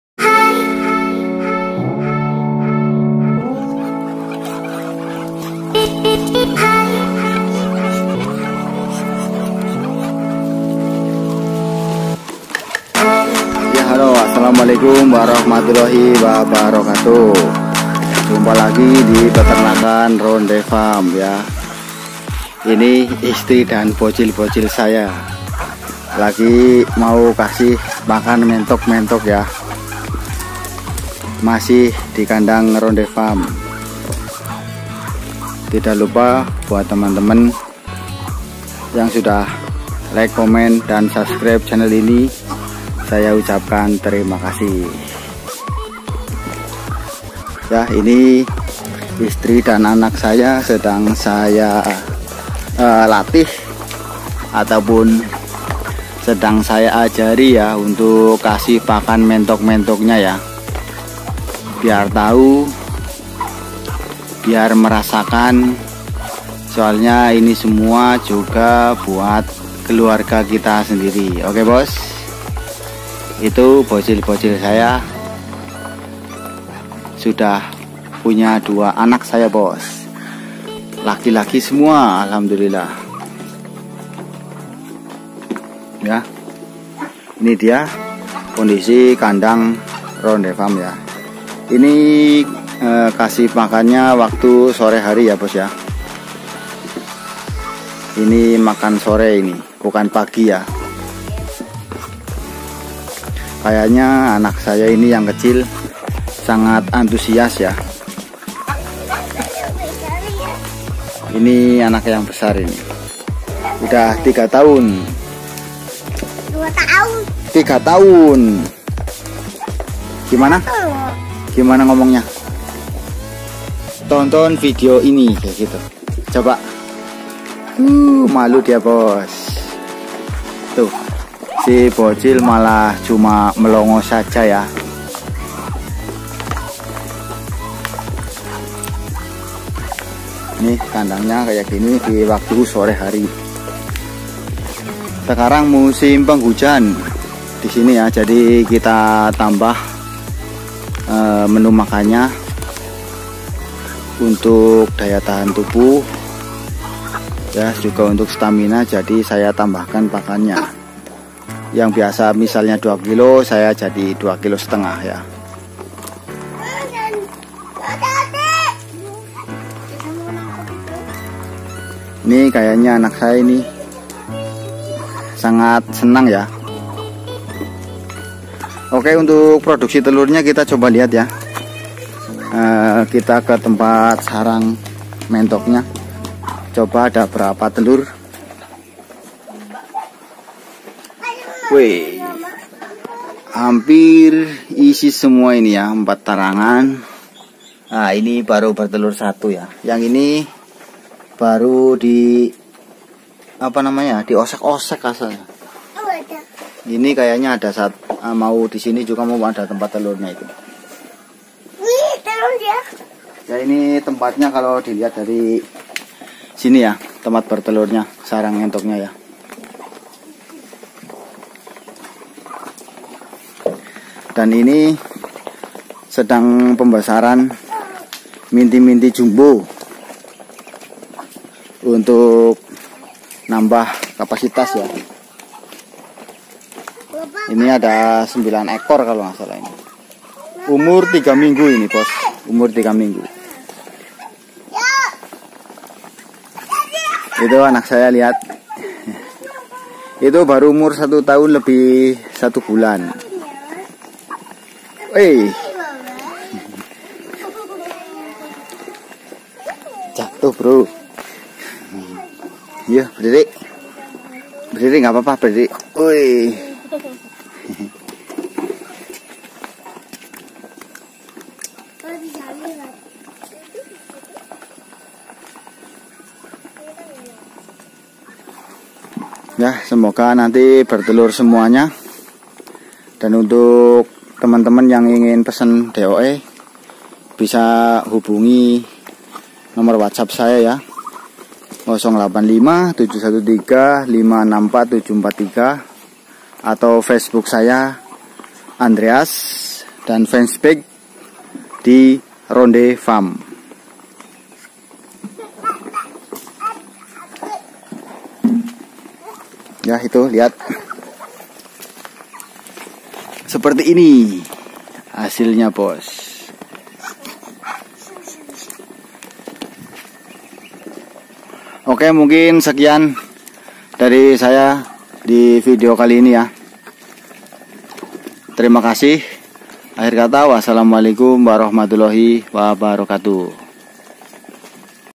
Kandang RONDE FARM mulai produksi di pantau anak istri
Sealamat sore bosq..
masih di kandang @Rondefarm  kali ini saya ajak anak istri untuk kasih pakan mentok2nya oke bagaimana keseruanya yuk kita simak sama sama
saling blejar dan saling suport sesama peternak terimakasih